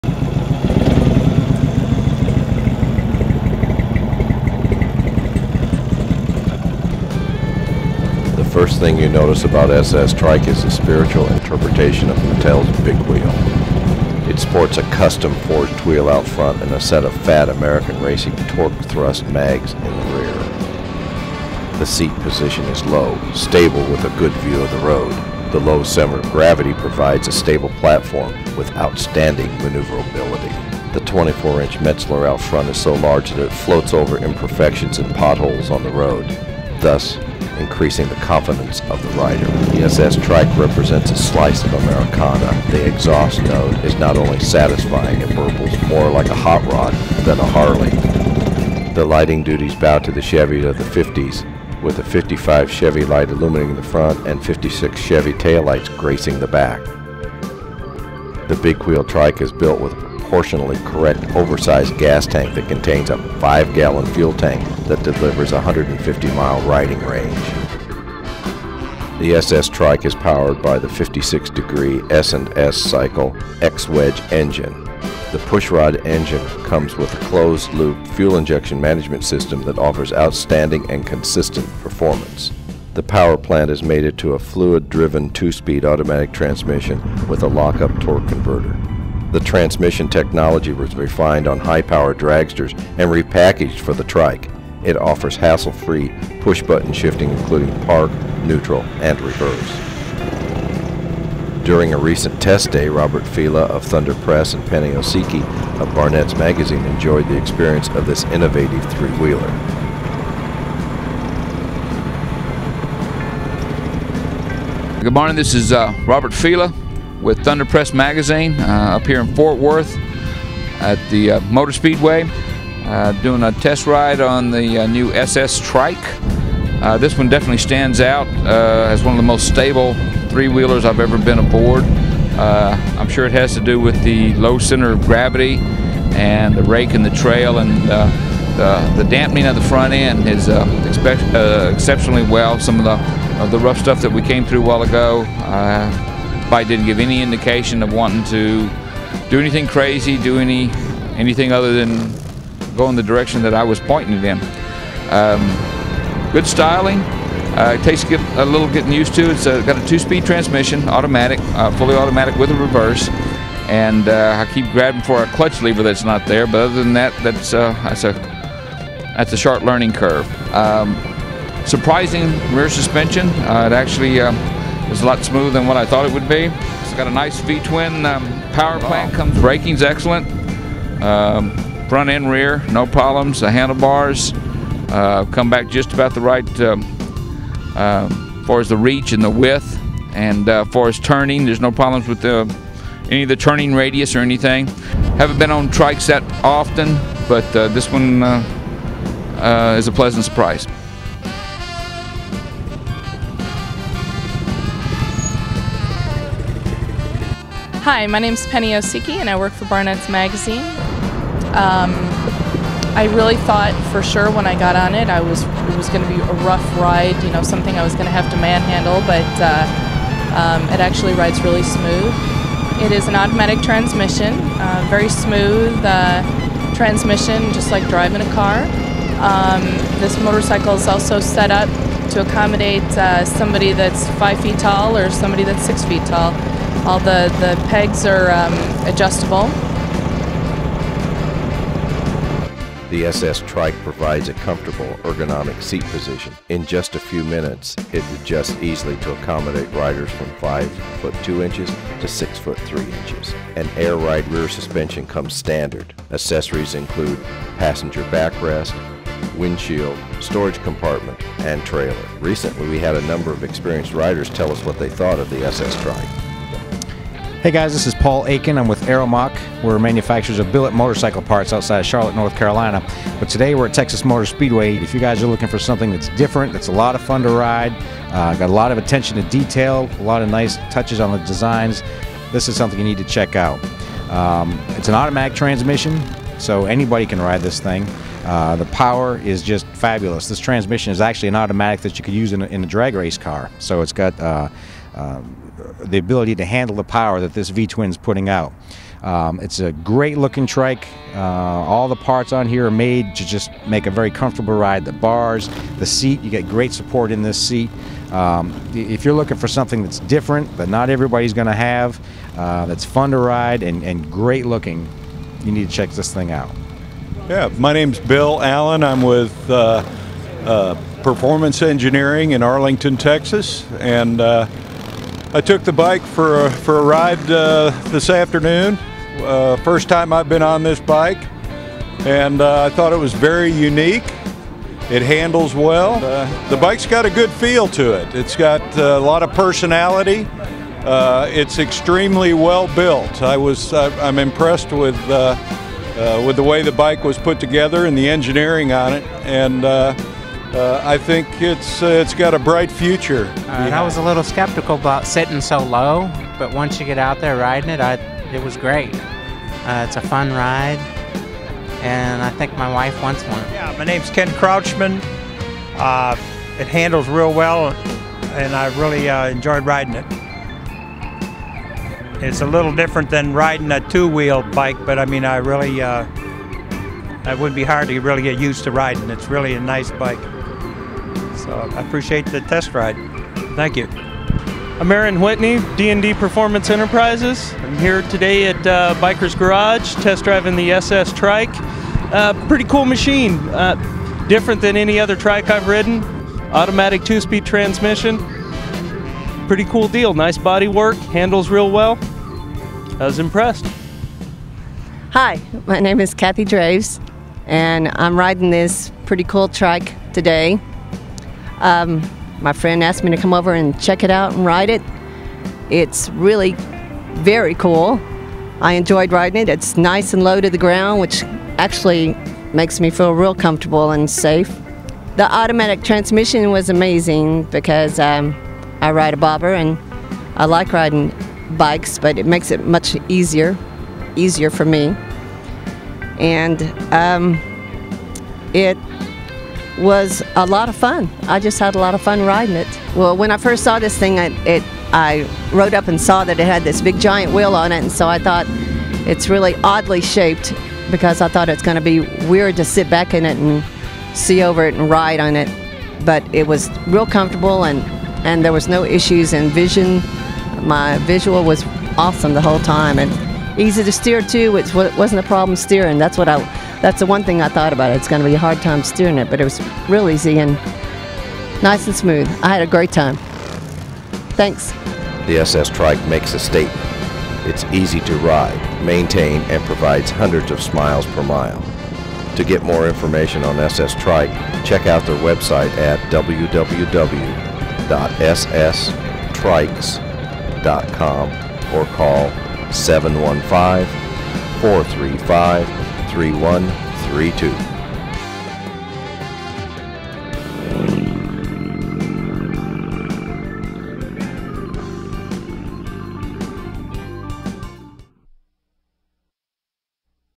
SS Trike First Show
The Big Wheel SS Trike made its North American debut on Saturday February, 20, 2010 at A Bikers Garage in Roanoke Texas. A number of enthusiast took test rides and this video records their impression of this innovative machine.